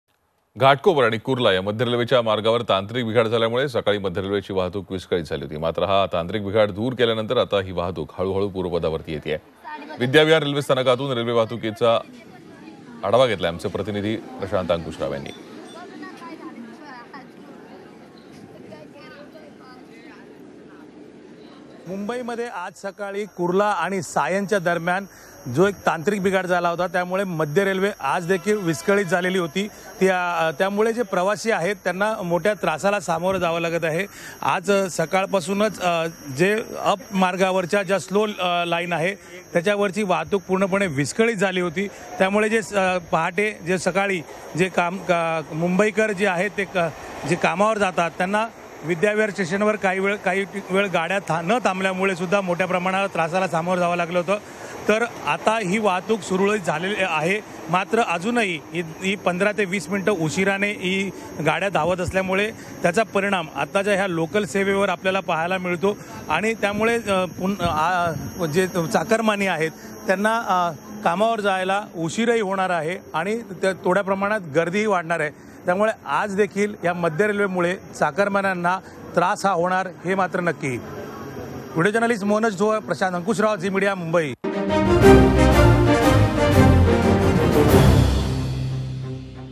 मुंबई | मध्य रेल्वे पूर्व पदावर; तांत्रिक बिघाड दूर
Mumbai,Vidya Vihar Central Railway Derail Due To Techniqual Issue Update At 10 PM